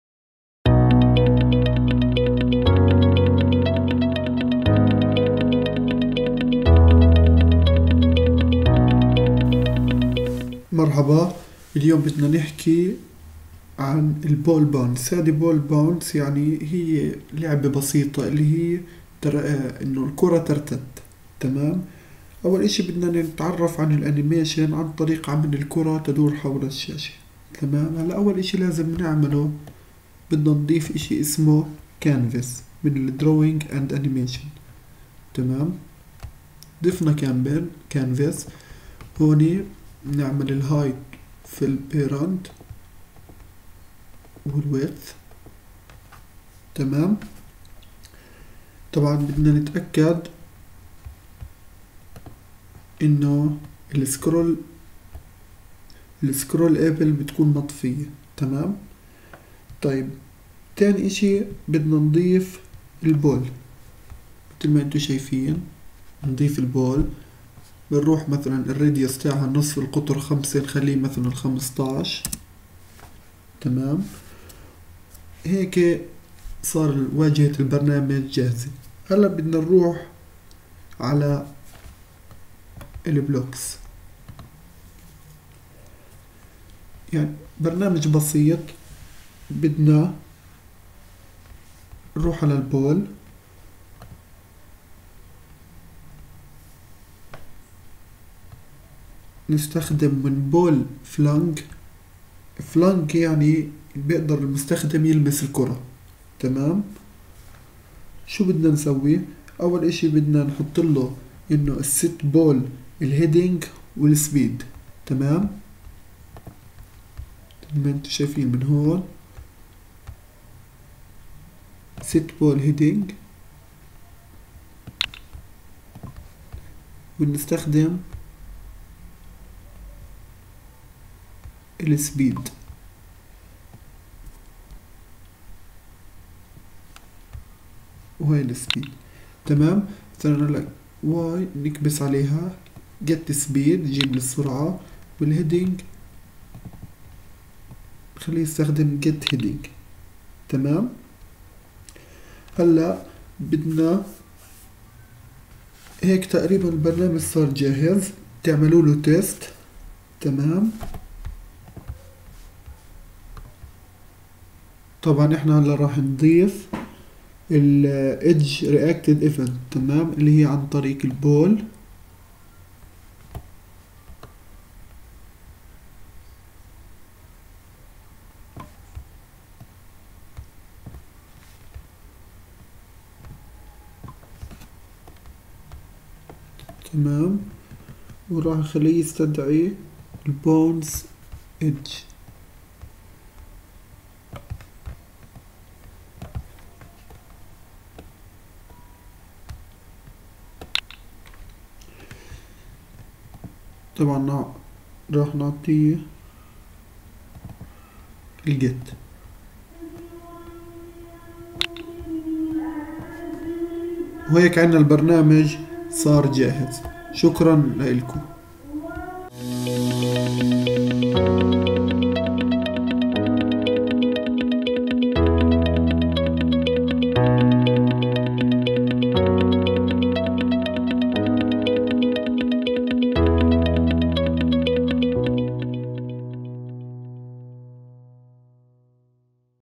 Technovation Palestine - Code - MIT App Inventor - ball bounce - Arabic Tutorial
Technovation Palestine

Video Lesson Series... with tips, tutorials, and more!

Ball bounce

Video by:
Johnney Majrouh

Supervised by:
Tina Hazboun

Technovation Palestine 2019 ©